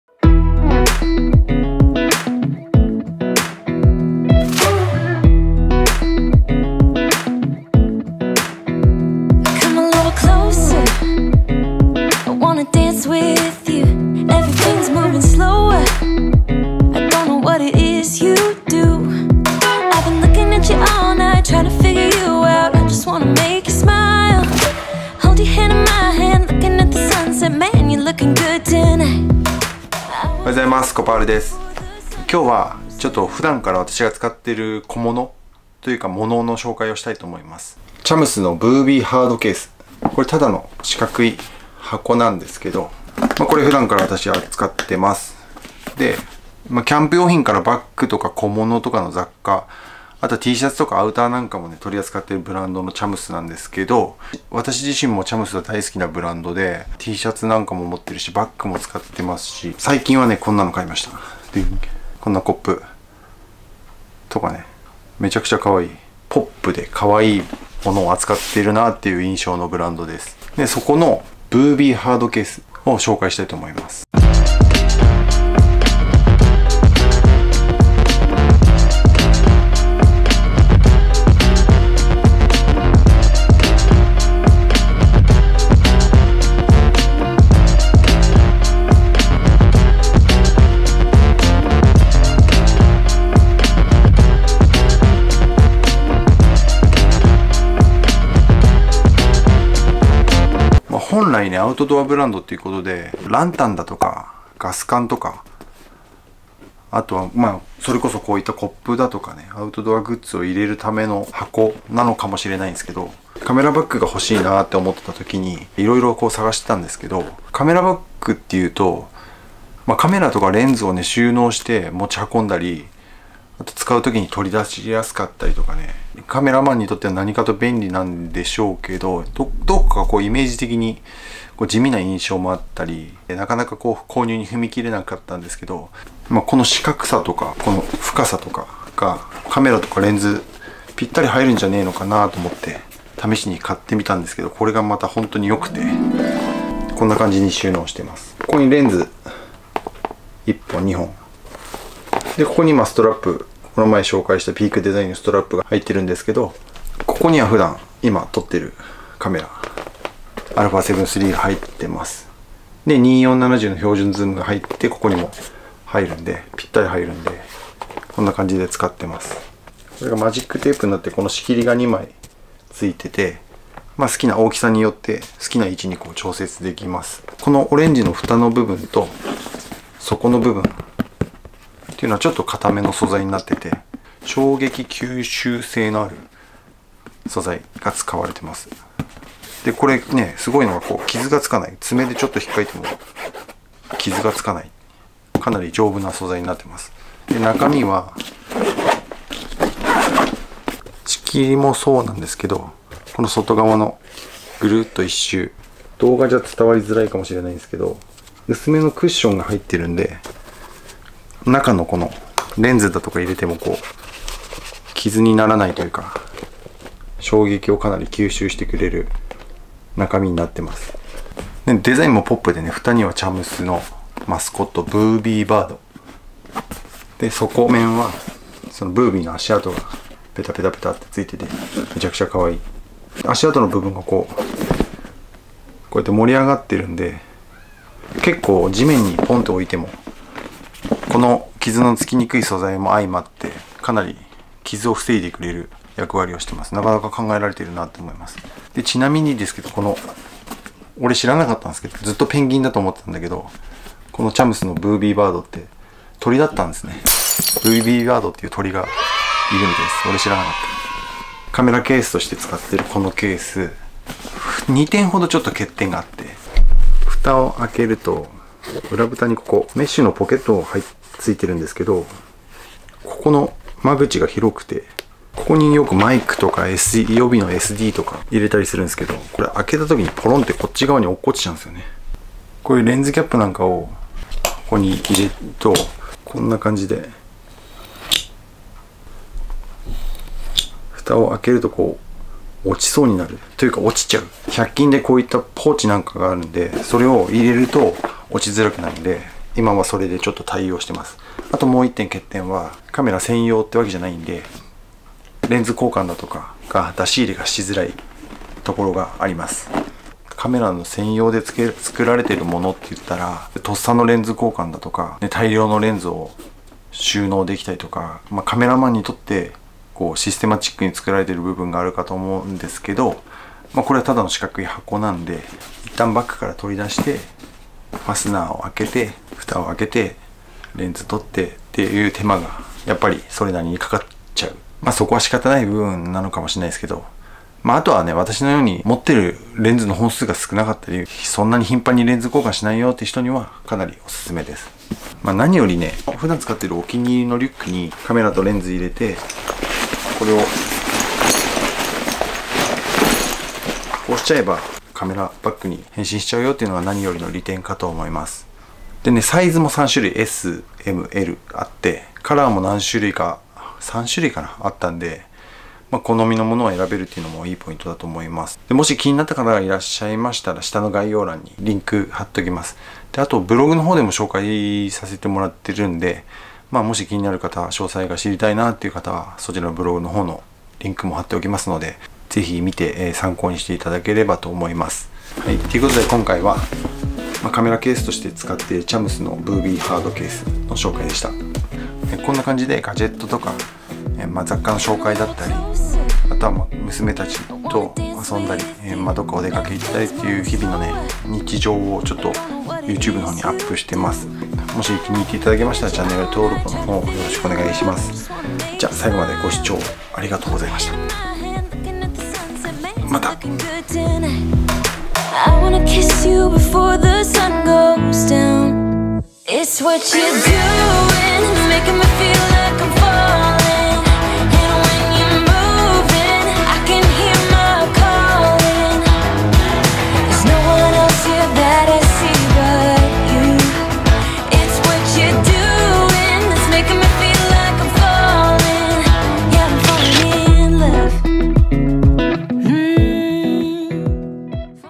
このケース最高!!【チャムス ブービーマルチハードケース】カメラやレンズの持ち運びにはコレ!!
【CHUMS】公式通販サイト

【ブービーマルチハードケース】amazonリンク

カジュアルなアウトドアグッズからバッグや雑貨、
Tシャツやアウターまで、ポップでオシャレなグッズを展開するブランド【CHUMS】

カメラの持ち運びにカメラバッグが欲しいなー、といろいろと物色していた所、
今回紹介するCHUMSのブービーマルチハードケースに出会いました。

丈夫でオシャレで機能性も良く、何より地味なモノが多い印象のカメラバッグではなく、
このマルチケースにカメラやレンズを収納して、普段使っているお気に入りのバッグに入れてしまえば、
カメラバッグに変身出来るのがとても良いです。